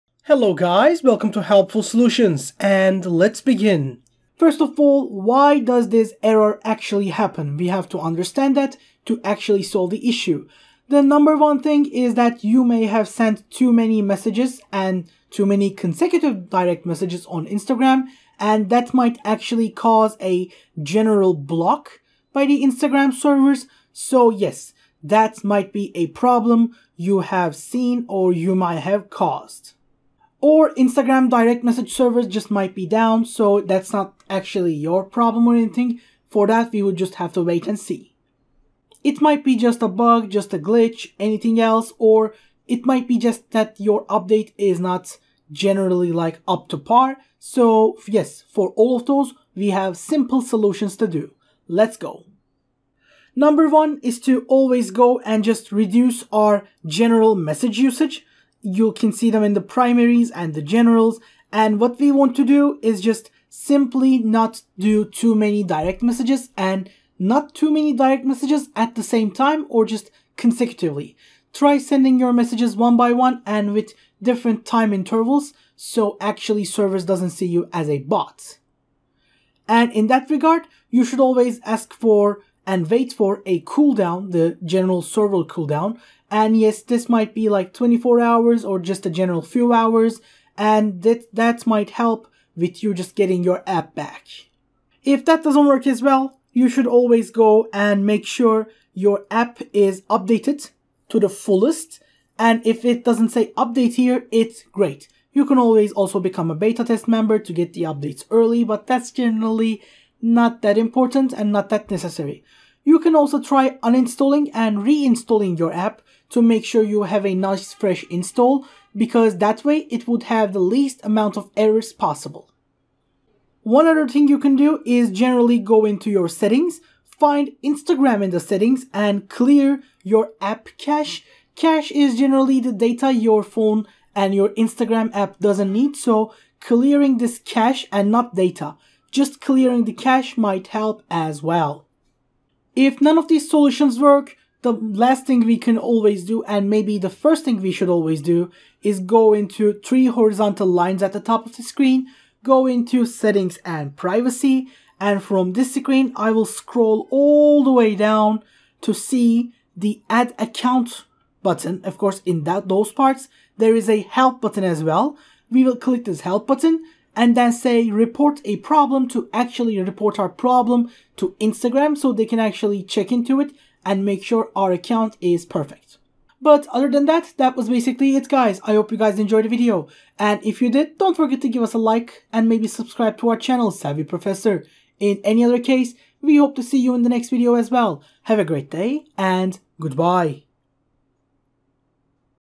How To Fix Couldn't Create Thread On Instagram (Instagram Couldn't Create Thread (How to Fix))
How To Fix Couldn't Create Thread On Instagram (Instagram Couldn't Create Thread (How to Fix)). In this tutorial I will show you how to fix coudln't create thread on instagram.